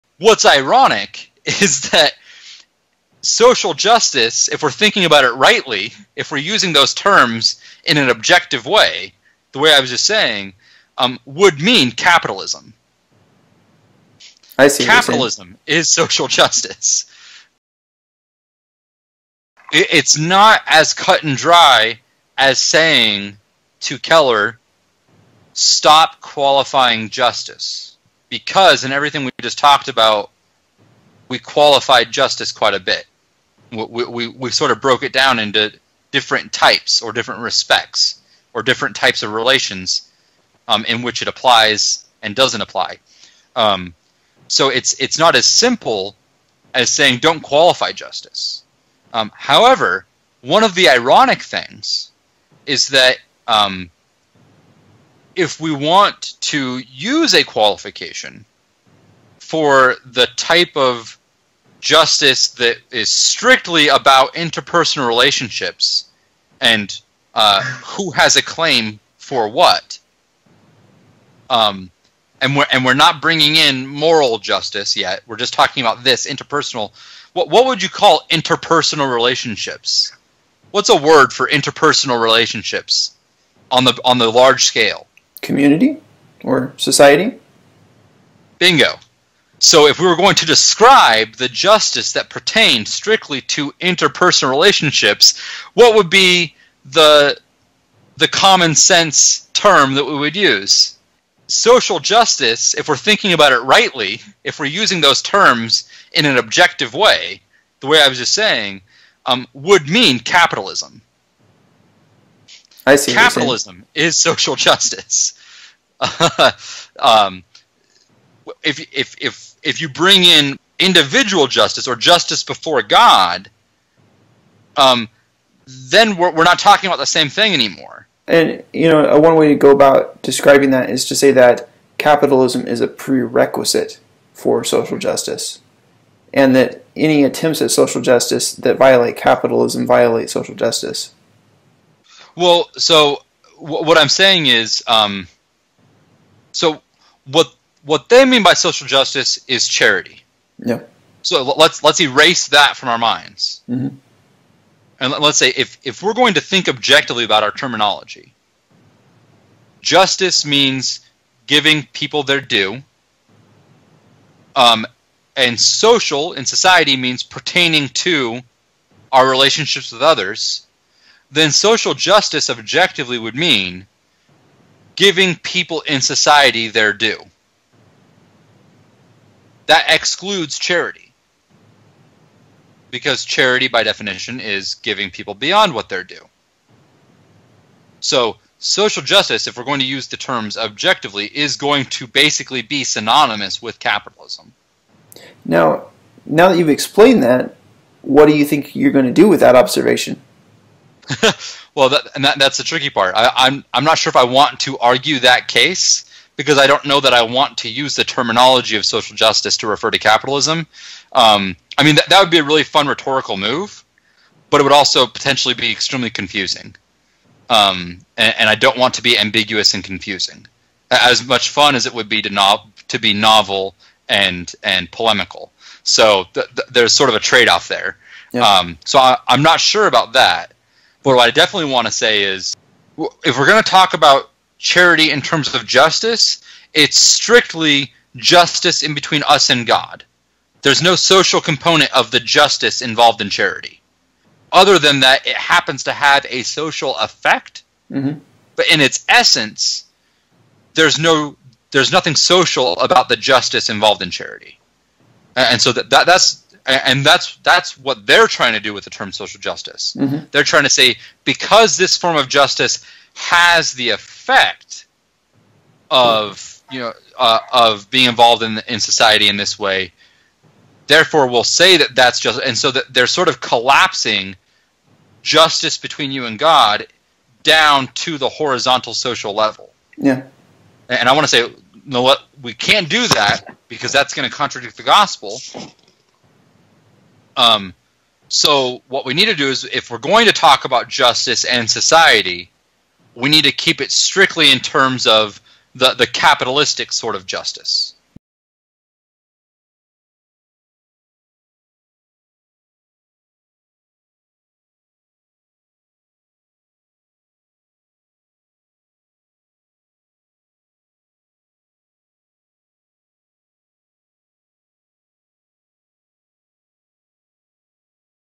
True "Social Justice" Is Capitalism
This is an excerpt from "Social Justice: Challenging Timothy Keller's 'Generous Justice'"